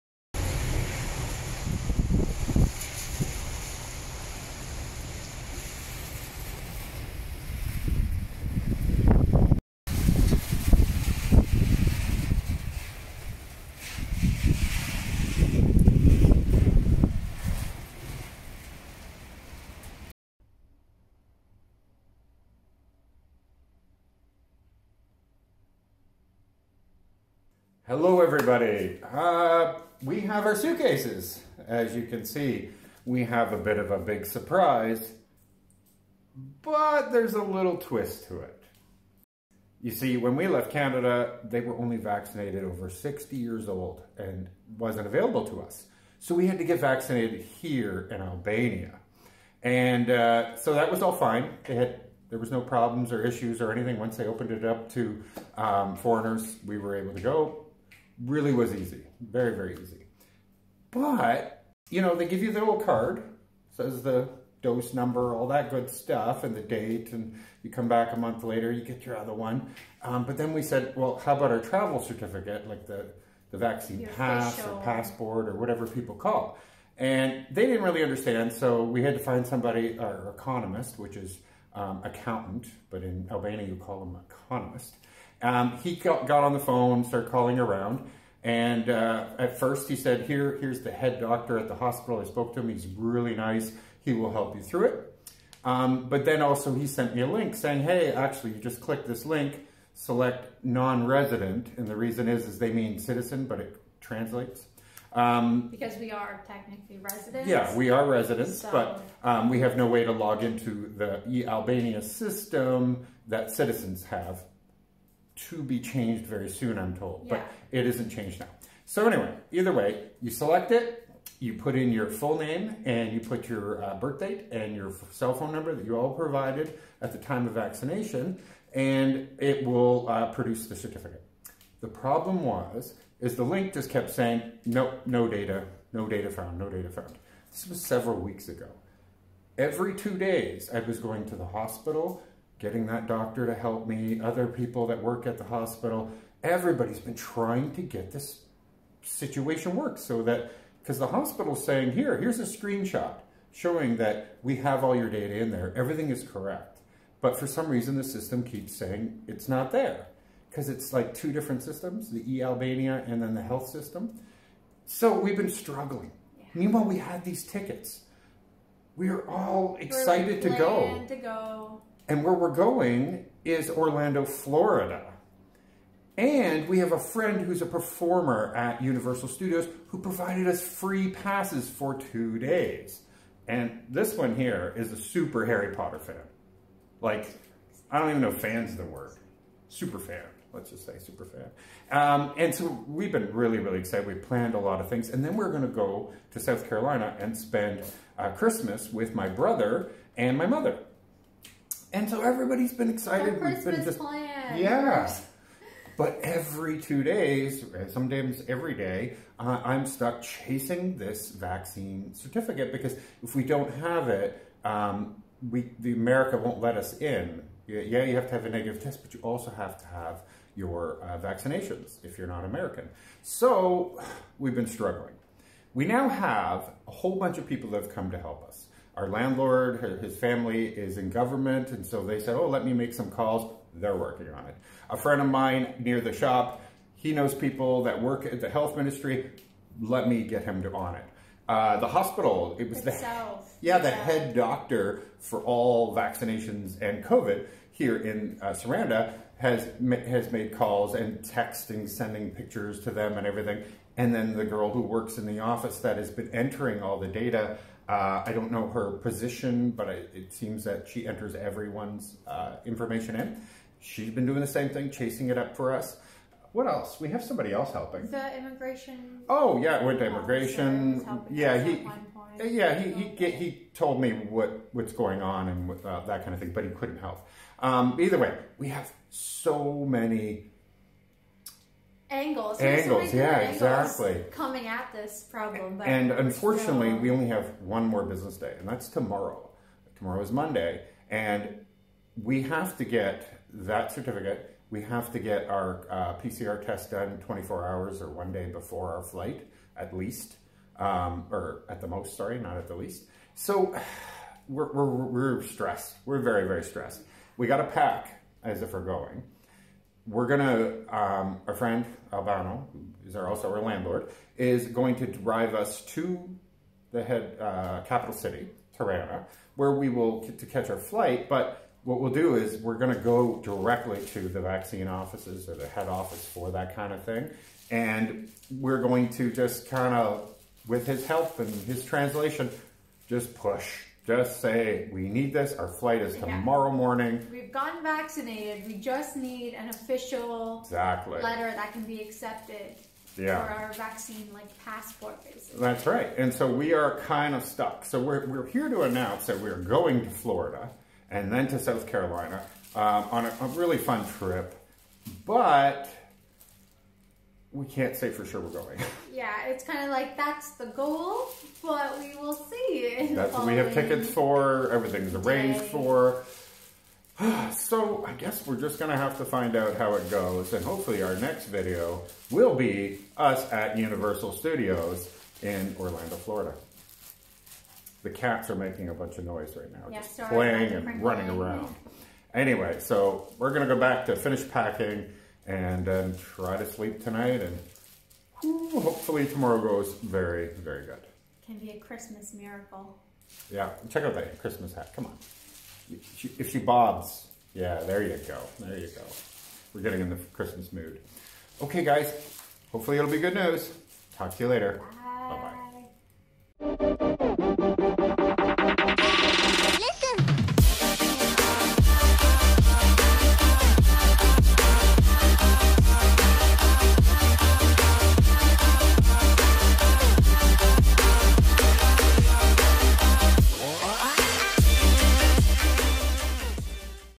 We Are Traveling Abroad For Christmas...Well Maybe? LoveTravelYT
Instagram is  @LoveTravelYT